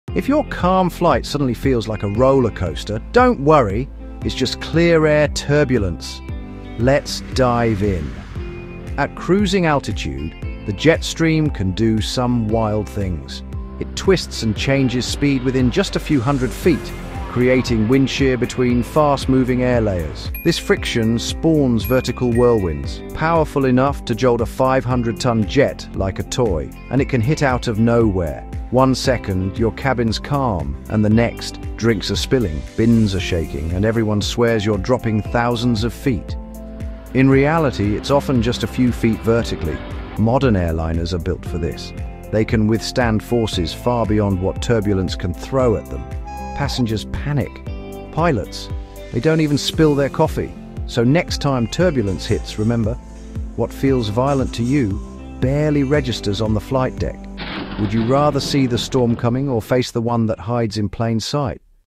Why does turbulence strike without warning on clear days?
The turbulence you’ll never see coming — clear-air turbulence can strike out of nowhere, even on a perfectly sunny day.